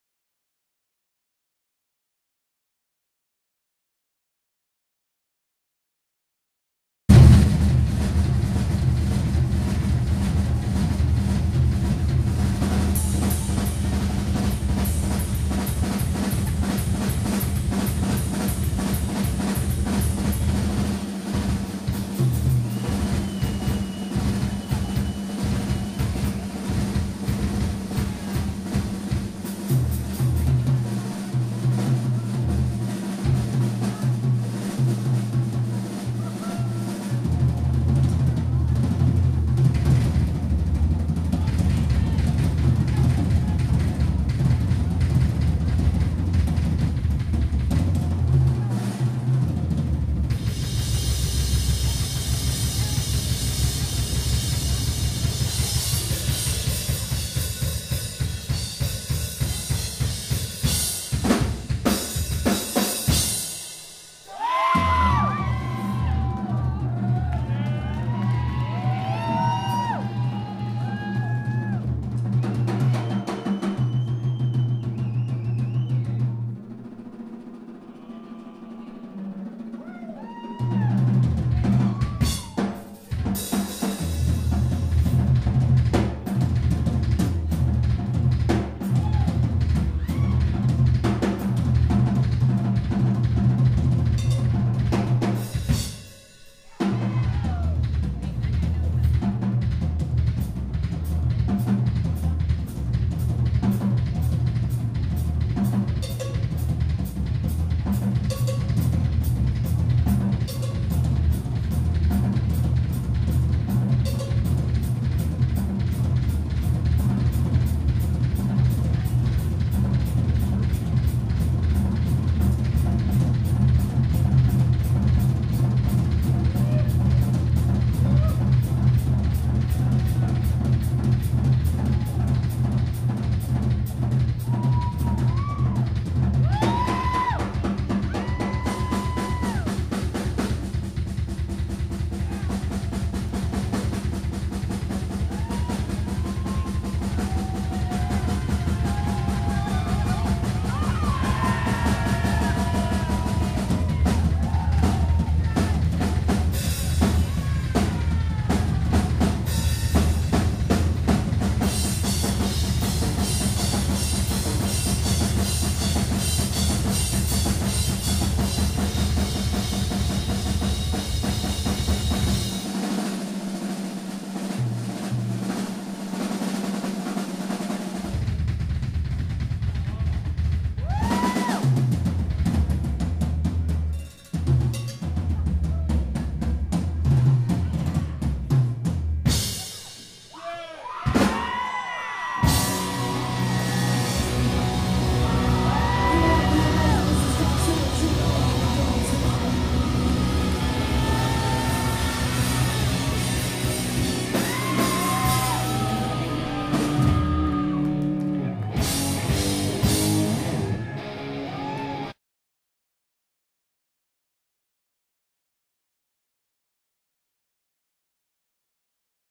Scott Churilla Drum Solo
Scott "Zilla" Churilla of the Supersuckers performs a spectacular drum solo at Exit In in Nashville TN.  Filmed by Jeremy Owsley